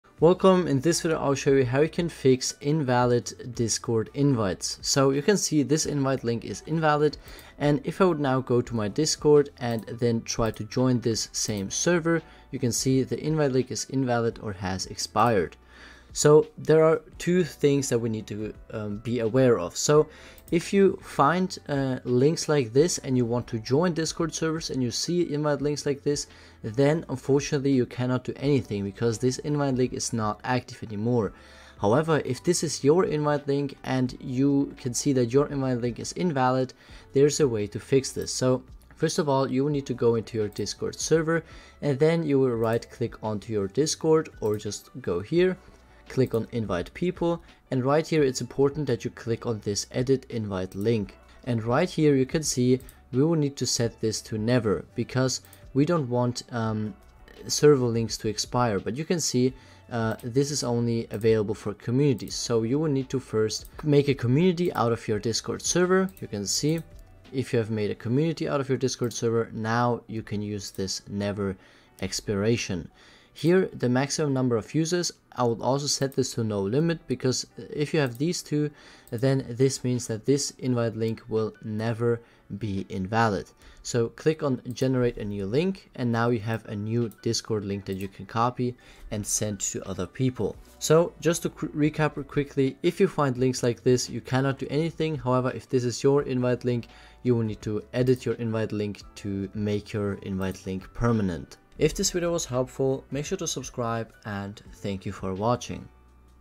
How To Fix Invalid Invite Link on Discord (WORKING 2025)
In this video I will show you how you can fix invalid invite links on discord.